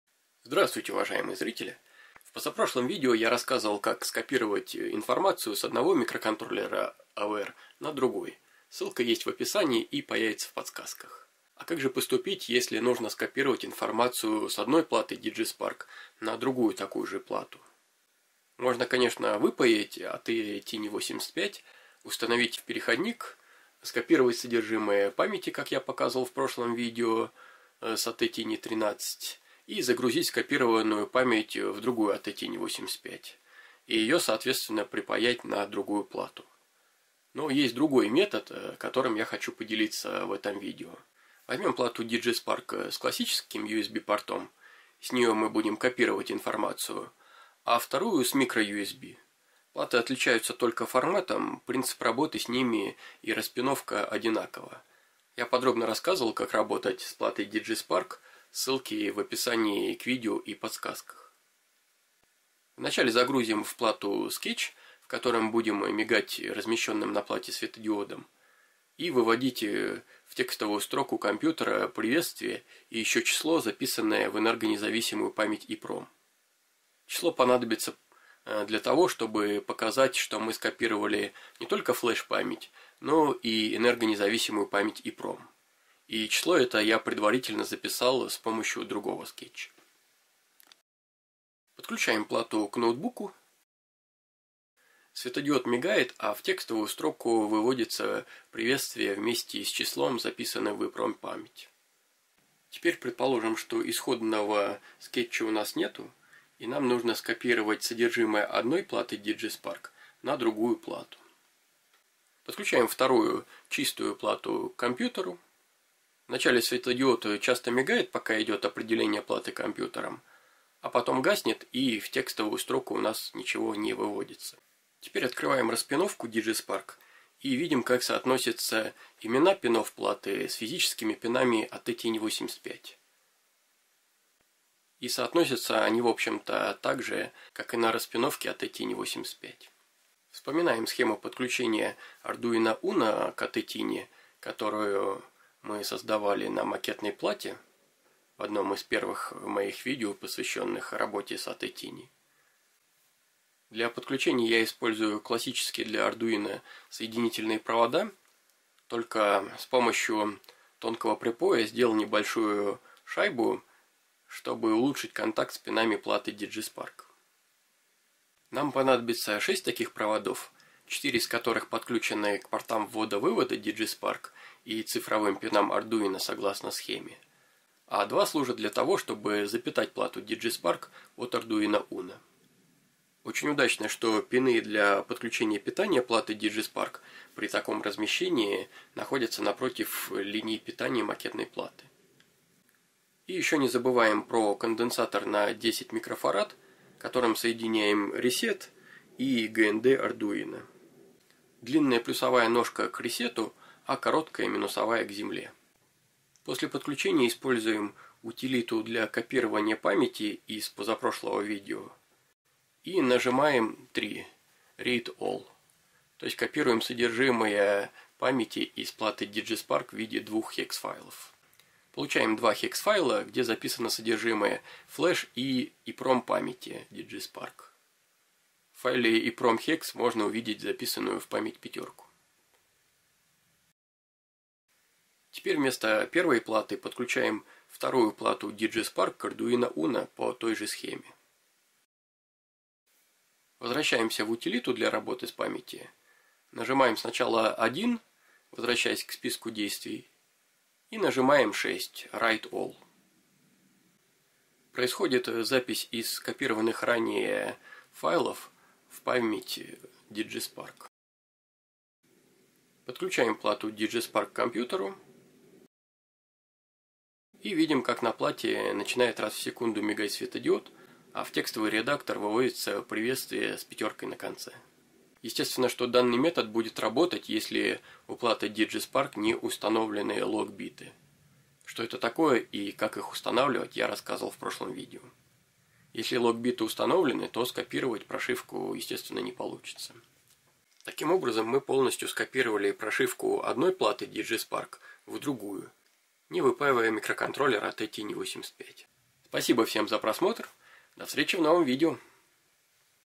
Копируем содержимое платы Digispark ATtiny85 в другую плату Digispark
В данном видео скопируем содержимое памяти одной платы Digispark ATtiny85 в другую плату Digispark. Операцию копирования произведем без выпаивания микроконтроллера ATtiny85 с платы Digispark.

В видео показано как скопировать не только flash память платы Digispark, но и содержимое энергонезависимой памяти EEPROM.

0:00 Как скопировать одну Digispark ATtiny85 в другую?
0:42 Загрузка скетча в первый Digispark
1:42 Подключение Digispark к Arduino UNO
3:26 Считывание прошивки Digispark 1
3:54 Копируем информацию на Digispark 2